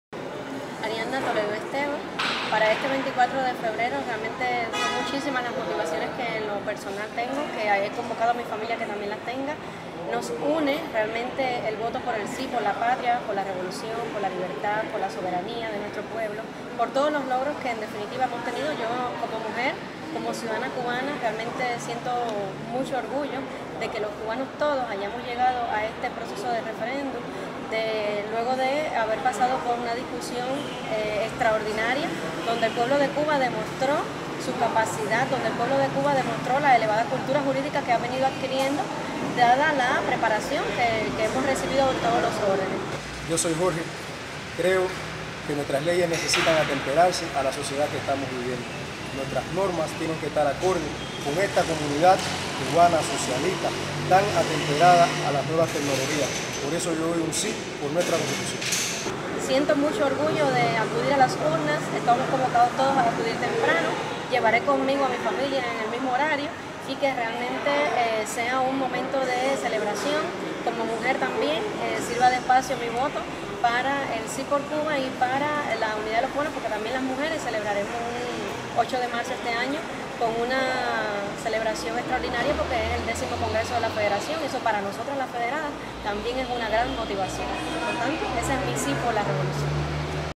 Santiagueros comprometidos por el SI
Jóvenes santiagueros expresan  las motivaciones que los comprometen a votar si por la Constitución el próximo 24 de Febrero